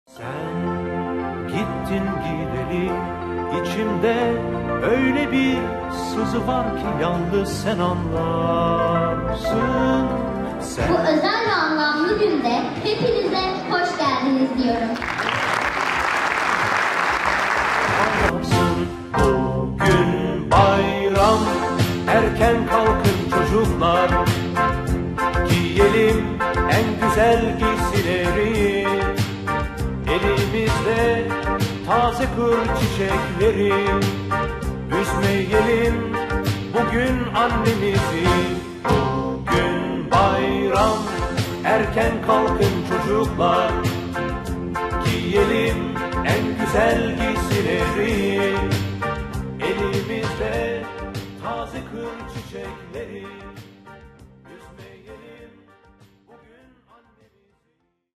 ‘nde 23 Nisan🎈
23Nisan Ulusal Egemenlik ve Çocuk Bayramı'nı Lösante Hastanemizde, lösemili kahramanlarımızla birlikte kutlamanın gururunu yaşıyoruz! Onların gücü ve azmi, bize her zaman umut ve ilham veriyor. Bugün ve her gün yanınızdayız çocuklar🇹🇷♥️